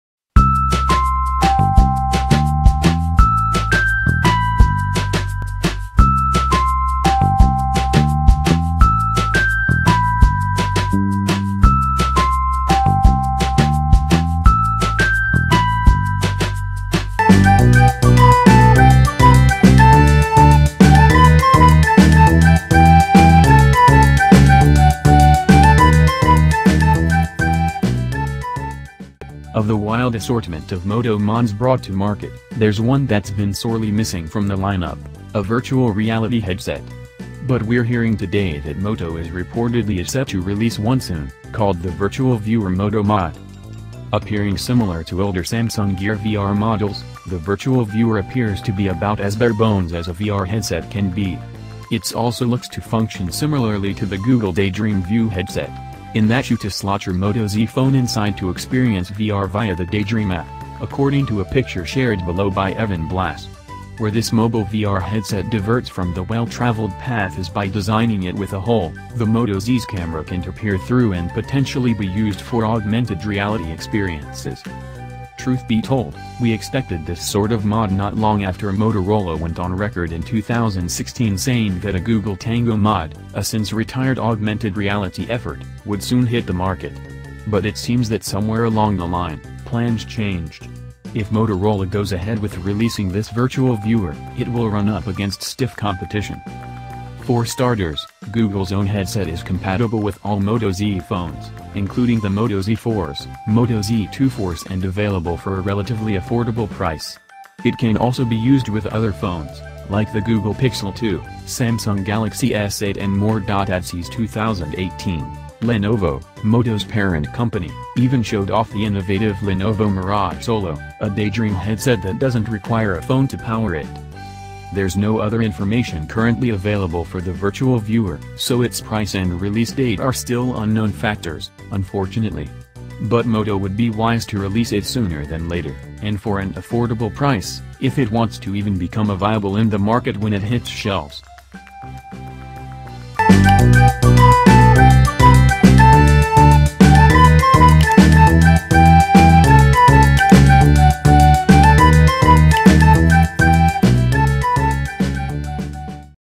AMAZING..!! Motorola makes VR (virtual reality) Headset.
Of the wild assortment of Moto Mods brought to market, there’s one that’s been sorely missing from the lineup: a virtual reality headset. But we’re hearing today that Moto is reportedly is set to release one soon, called the Virtual Viewer Moto Mod.

Appearing similar to older Samsung Gear VR models, the Virtual Viewer appears to be about as barebones as a VR headset can be. It's also looks to function similarly to the Google Daydream View headset, in that you to slot your Moto Z phone inside to experience VR via the Daydream app, according to a picture shared below by Evan Blass.
SUBSCRIB NOW!

by High Technology